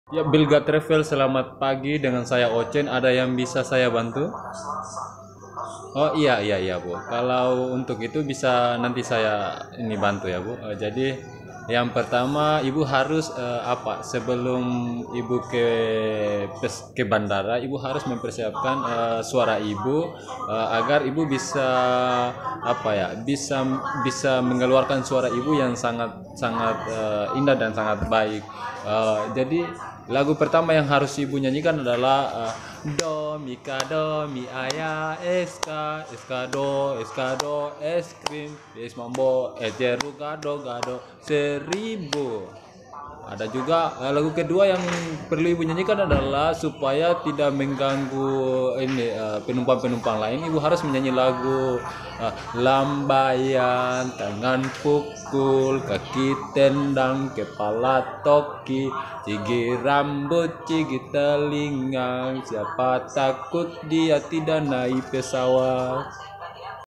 Komunikasi yang lucu antara costumer dan pegawai lion parcel
Komunikasi  lucu antara pegawai lion parcel dan costumer
Ditanya tiket malah jadinya bernyanyi